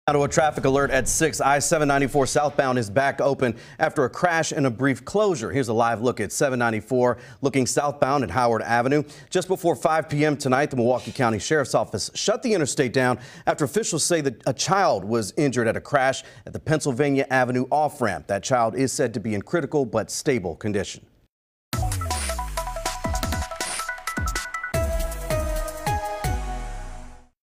I-794 reopens after crash leaves child critically injured
A child is critically injured in a vehicle crash on SB I-794, causing a full freeway closure. Traffic had to be diverted as first responders attended the scene.